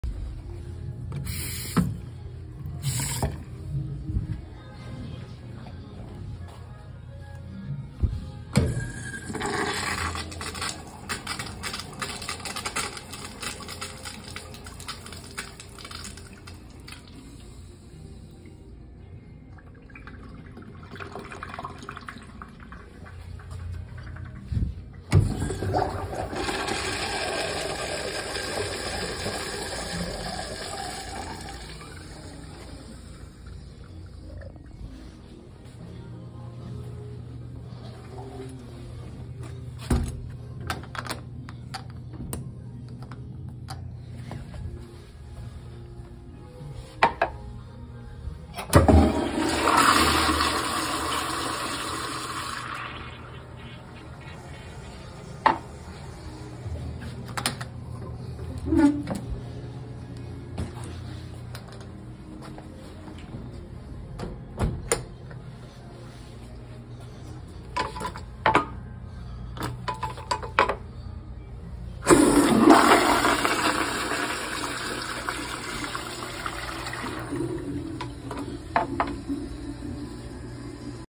Wok King international Buffet Men’s Restroom
2000s & 2020s
2 Kohler Dexter Urinals
1 AS Pressure Assisted Cadet Toilet
1 Newer AS Pressure Assisted Cadet Toilet
7011 W Canal Dr in Kennewick WA